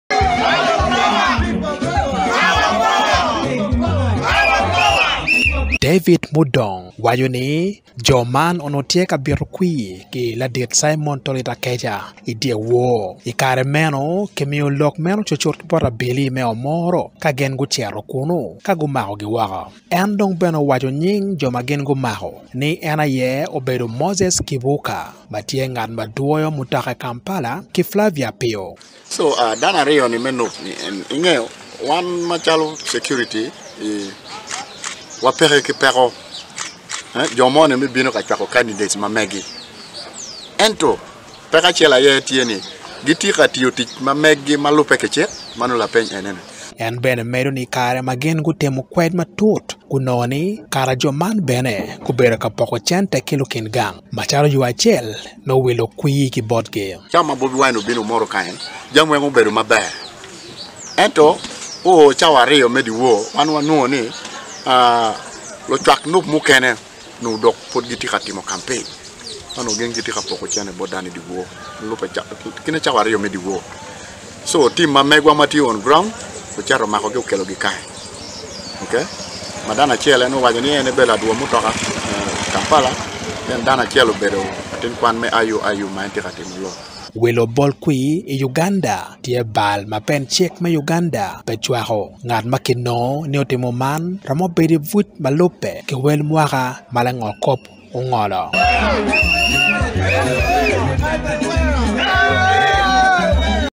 Kimako Dano Aryo Omoro I 2 Arrested in Omoro over election Fraud
Nen